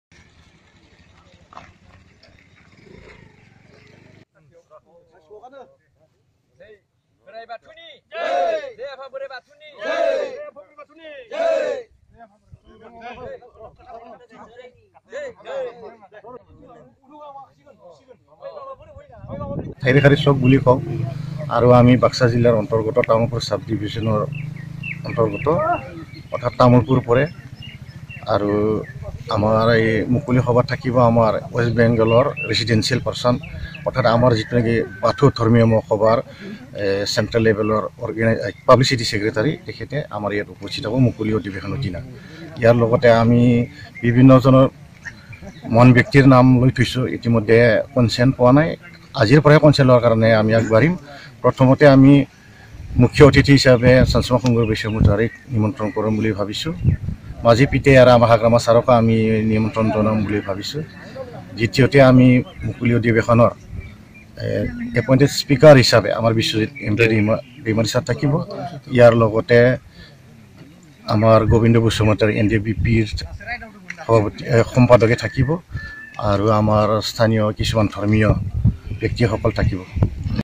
বাক্সাত তামুলপুৰ হিৰিম্বা বাথৌ মহোৎসৱৰ লাইখুঁটা স্থাপন
বাক্সা জিলা অন্তৰ্গত তামুলপুৰ মহকুমাত আগন্তুক ২৬ জানুৱাৰীৰ পৰা ১ ফেব্ৰুৱাৰীলৈ ৭দিনীয়া বিস্তৃত কাৰ্যক্ৰমণিকাৰে পঞ্চম বাৰ্ষিক বাথৌ মহোৎসৱ উদযাপন কৰাৰ ব্যাপক প্ৰস্তুতি চলোৱা হৈছে ৷ তদুপলক্ষে আজি  টি এল চি চিৰ অধ্যক্ষ  লেবেন চন্দ্ৰ বড়োই  বাথৌ ধৰ্মৰ ৰীতিনীতিৰে থাইগিৰখাৰিৰ বাথৌ ফৌথাৰত লাইখুঁটা স্থাপন কৰে ।